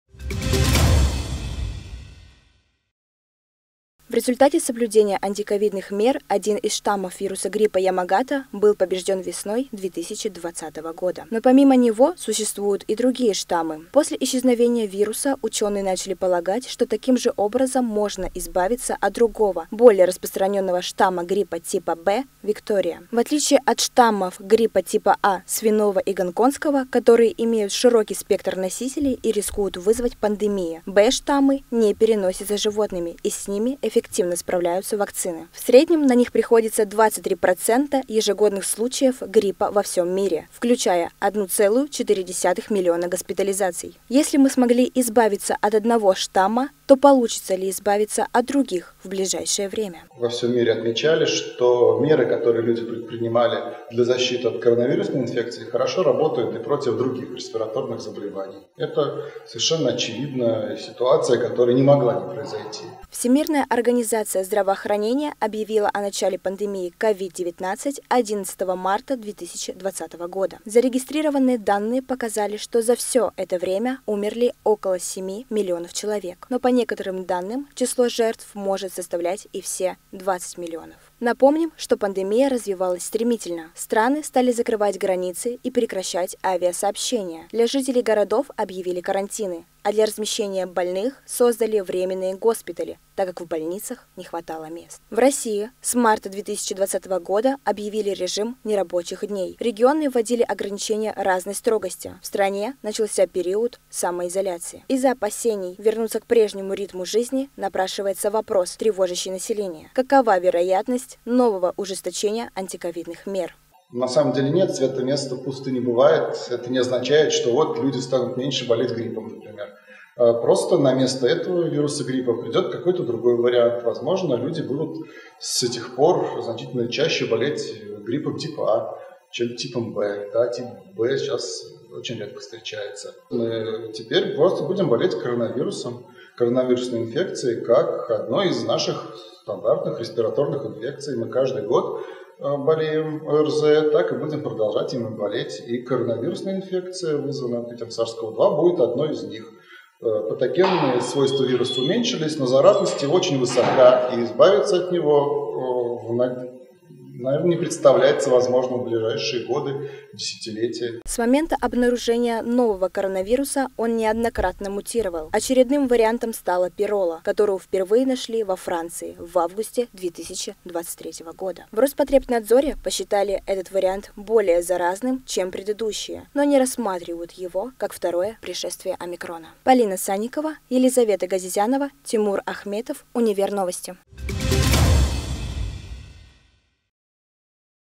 ШТАММ ГРИППА ВЫМЕР ИЗ ЗА ПАНДЕМИИ КОРОНАВИРУСА
Самоизоляция, ношение масок и другие меры, направленные на сдерживание распространения COVID-19, привели к исчезновению одного из самых распространенных штаммов вируса гриппа B – Yamagata («Ямагата»). Сегодня учёные выявляют только три штамма, а «Ямагата» не наблюдается с марта 2020 года.